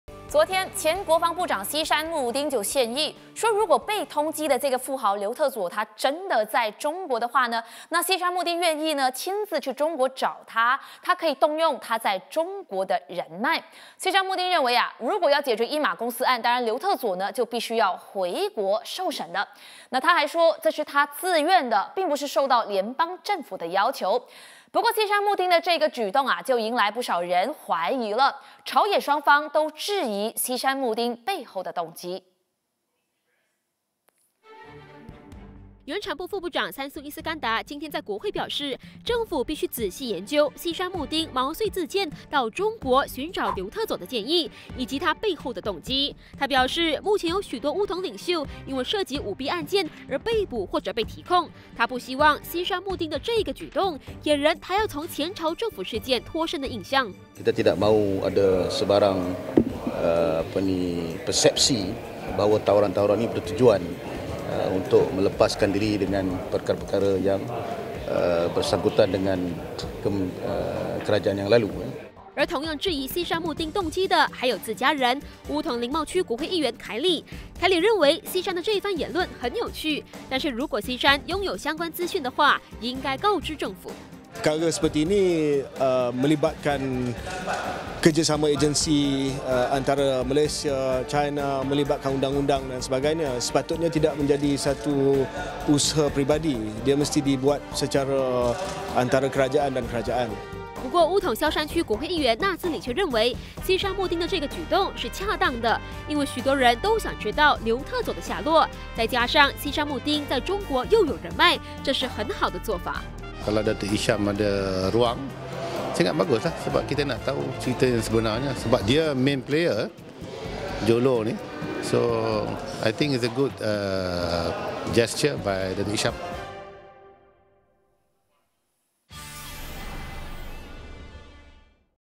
Prime Talk 八点最热报 23/10/2018 - 三苏: 政府将研究希山献议寻找刘特佐动机
前国防部长希山慕丁昨天主动献议，愿意亲自到中国寻找刘特佐。原产部副部长三苏依斯干达表示，不想给人觉得这是希山企图逃离前朝政府事务的印象，政府会仔细研究希山的动机。